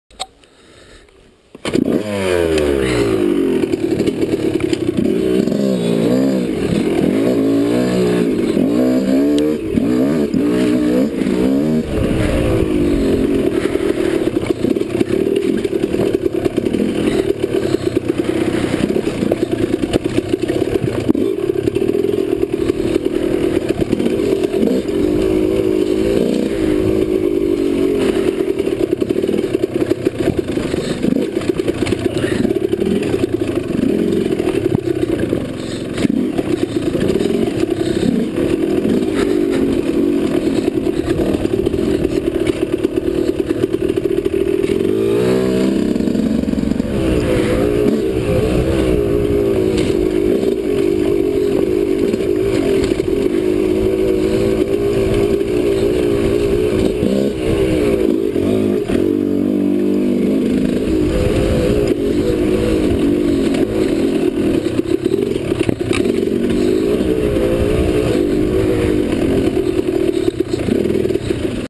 Foresthill lower #5 with Jon
Small section of lower trail #5 at Foresthill with Jon